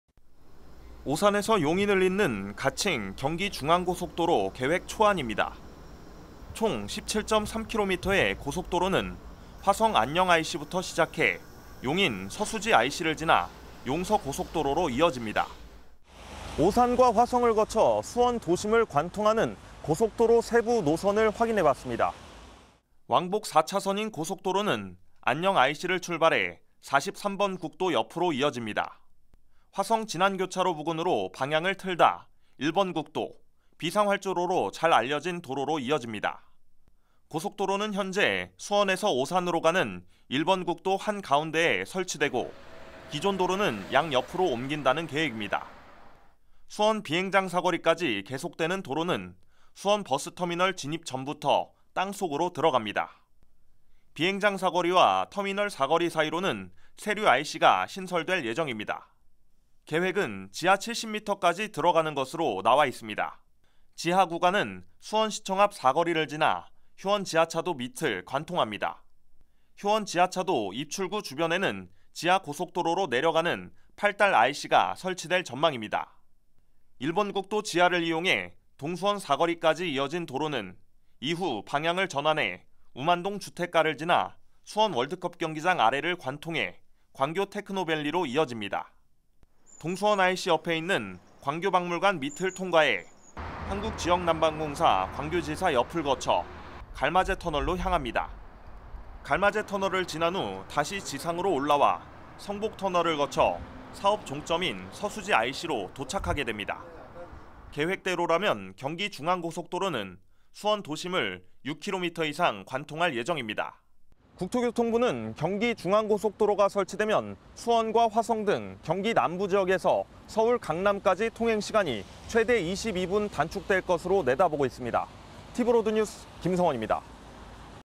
[수원]수원・화성 지역 ‘논란 예고’/티브로드
오산에서 용인을 잇는 민자 고속도로 계획이 발표되면서
수원과 화성지역에 논란이 예상되고 있습니다.
수원 시내를 지하로 관통할 예정인데요.
오늘은 이 오산용인고속도로에 대해 집중 보도해드립니다.
총 17km에 달하는 이 도로의 노선을 상세히 전해드립니다.

-티브로드수원